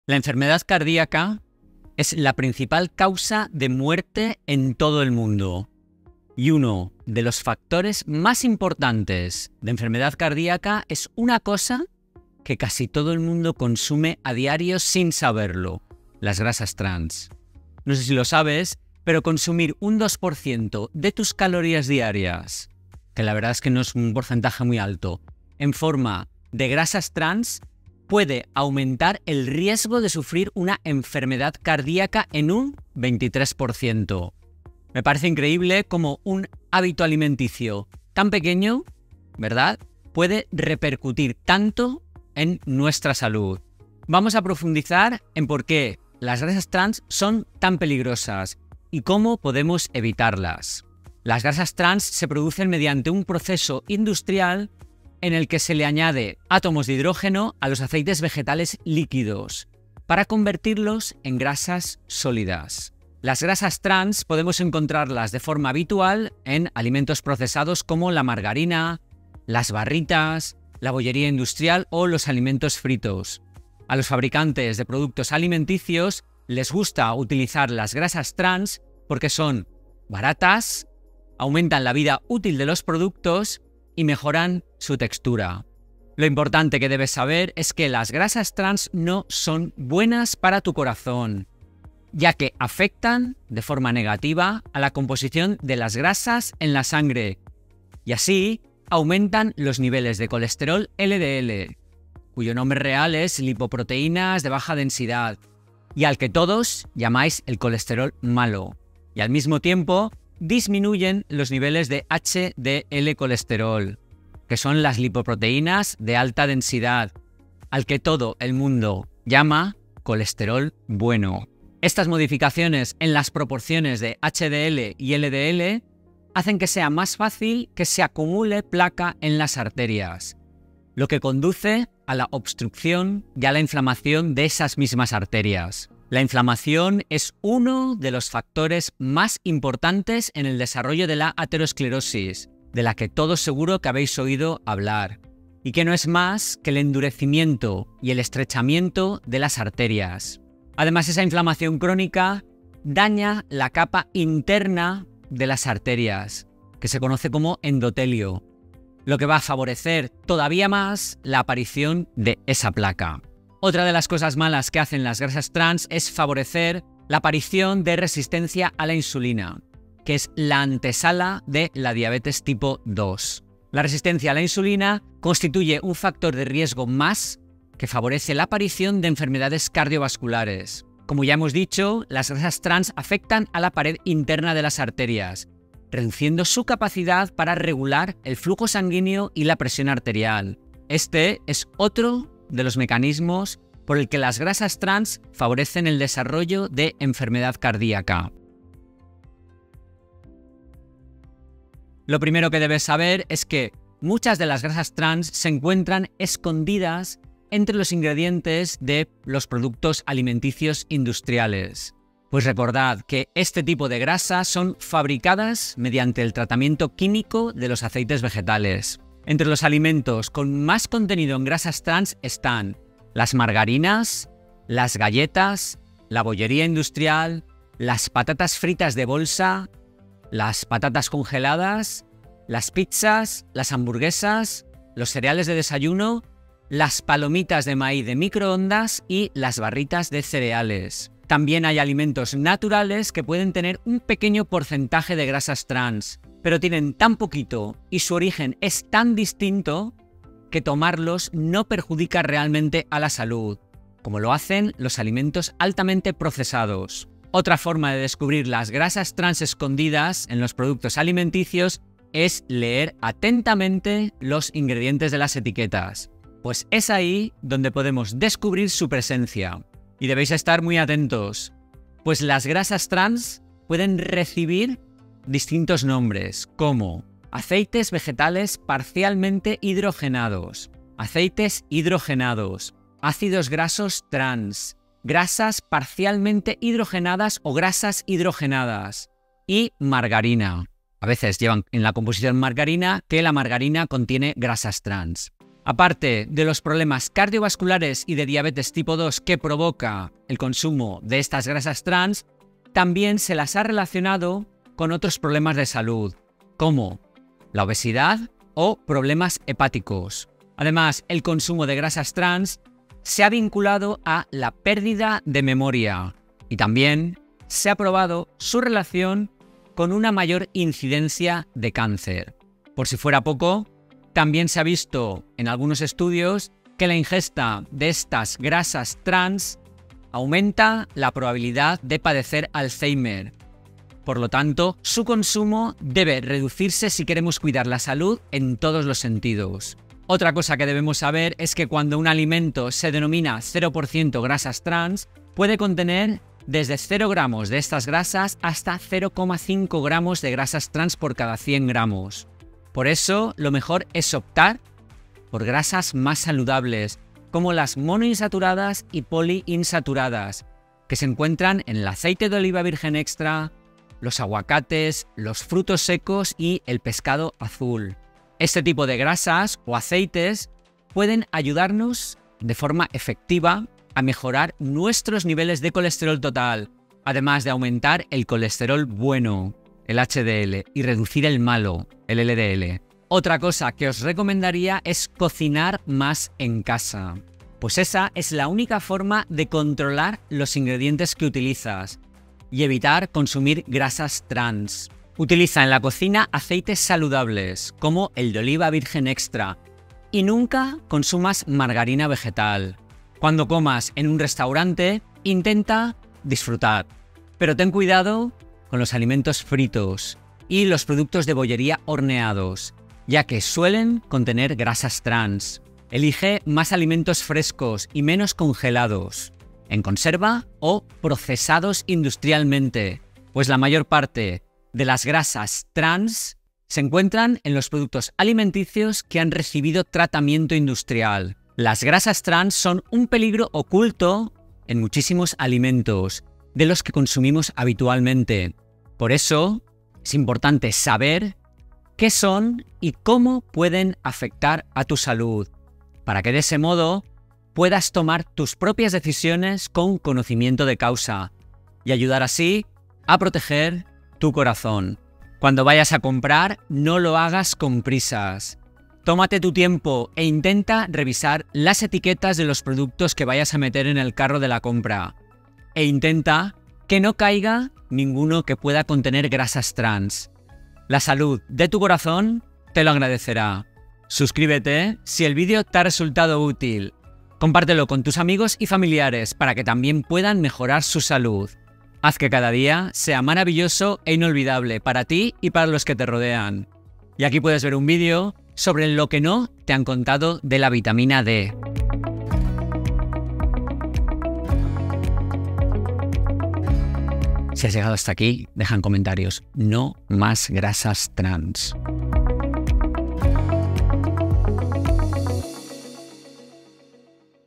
El asesino OCULTO del CORAZÓN ¡la única comida que debes evitar!
Descubre cómo las grasas trans están destruyendo tu salud y aumentando tu riesgo de enfermedad cardíaca. Descubre qué alimentos evitar y cómo proteger tu corazón.

00:00 El asesino oculto del corazón ¡la única comida que debes evitar!
00:44 ¿Qué Son las Grasas Trans y Por Qué Son Tan Peligrosas?
01:03 Alimentos Comunes que Contienen Grasas Trans
01:25 Impacto de las Grasas Trans en el Cuerpo
03:18 Cómo Identificar las Grasas Trans en los Productos Alimenticios
04:55 Otros Problemas de Salud Asociados con las Grasas Trans
05:52 Alternativas Saludables a las Grasas Trans
06:22 Consejos para Reducir el Consumo de Grasas Trans

La enfermedad cardíaca es la principal causa de muerte en todo el mundo, y hay un enemigo oculto en tu cocina que podrías estar consumiendo todos los días sin darte cuenta. En este video, te revelo el asesino oculto del corazón: las grasas trans. Te sorprenderás al saber que consumir solo un 2% de tus calorías diarias en forma de grasas trans puede aumentar el riesgo de enfermedades cardíacas en un 23%.

¿Qué hace que las grasas trans sean tan peligrosas? Te explico cómo este tipo de grasa afecta la salud de tu corazón al aumentar el colesterol "malo" (LDL) y reducir el "bueno" (HDL), contribuyendo a la formación de placa en las arterias, la inflamación crónica y la aterosclerosis. Además, las grasas trans pueden aumentar la resistencia a la insulina, lo que también incrementa el riesgo de desarrollar diabetes tipo 2, otro factor clave en las enfermedades cardiovasculares.

En este video, aprenderás a identificar las grasas trans escondidas en alimentos procesados como la margarina, la bollería industrial, los alimentos fritos y más. También te doy consejos para evitarlas y optar por alternativas más saludables como las grasas monoinsaturadas y poliinsaturadas, presentes en el aceite de oliva, los frutos secos y el pescado azul.

Cocinar en casa con ingredientes frescos y saludables es una de las mejores maneras de proteger tu corazón y prevenir problemas a largo plazo. Además, te explico cómo leer etiquetas de productos y qué términos buscar para identificar grasas trans ocultas.

Información legal.
Vídeo de grabación propia.